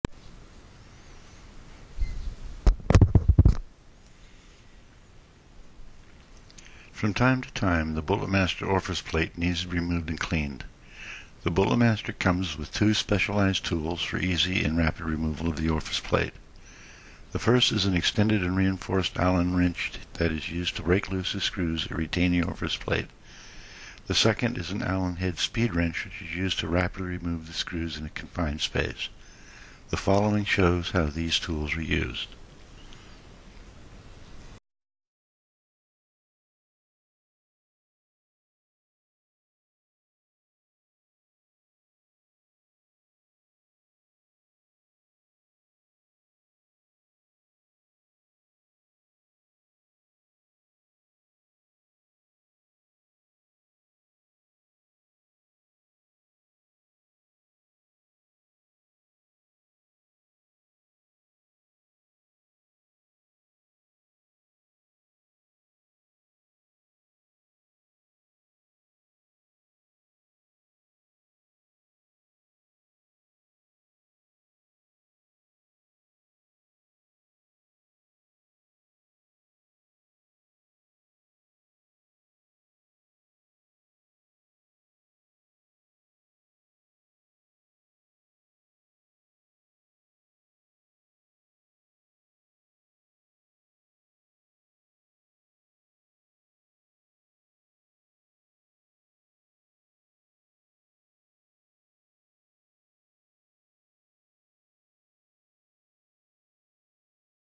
How to remove a Bullet Master Orifice Plate
How to remove a Bullet Master Orifice Plate using the specialized tools supplied with the Bullet Master.